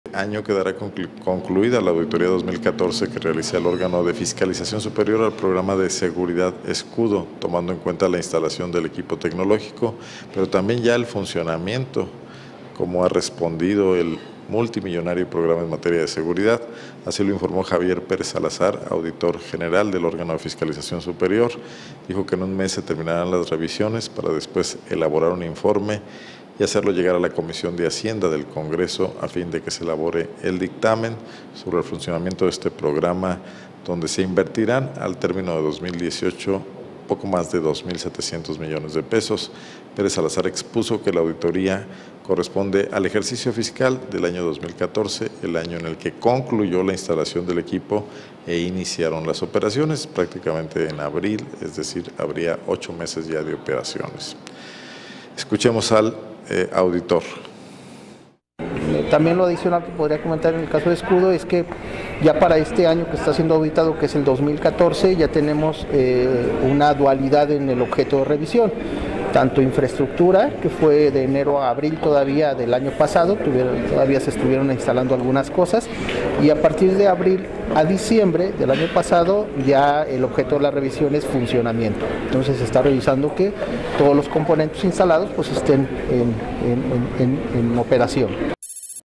Revisa OFS funcionamiento del programa de seguridad Escudo
En el segundo semestre del año quedará concluida la auditoría 2014 que realiza el Órgano de Fiscalización Superior al programa de seguridad Escudo, tomando en cuenta la instalación del equipo tecnológico, pero también el funcionamiento que ha dejado el millonario programa en materia de seguridad.